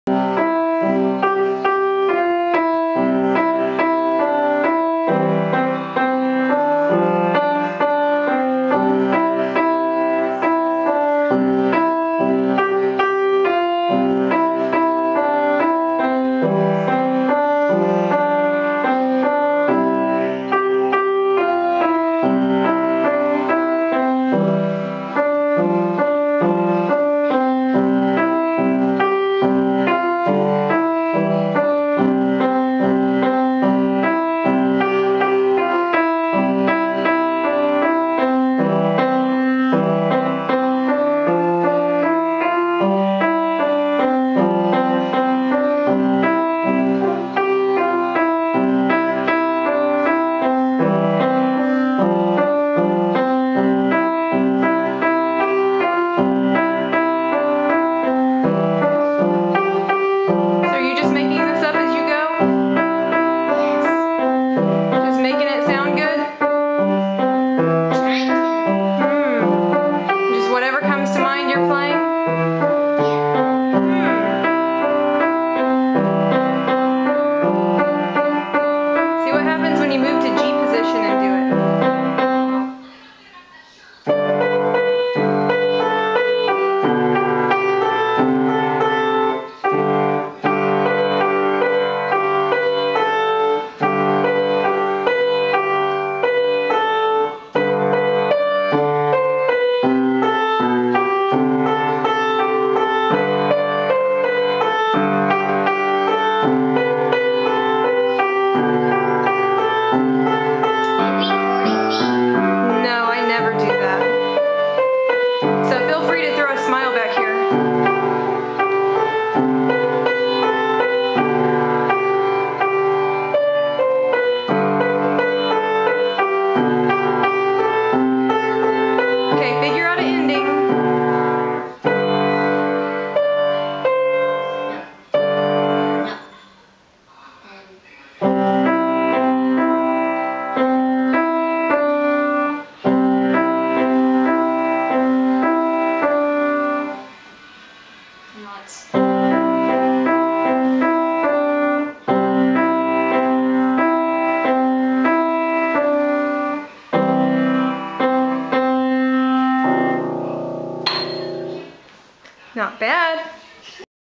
Piano Practice 2011 Spring Improv
Jenna has made up her own song!  Not bad for a rookie!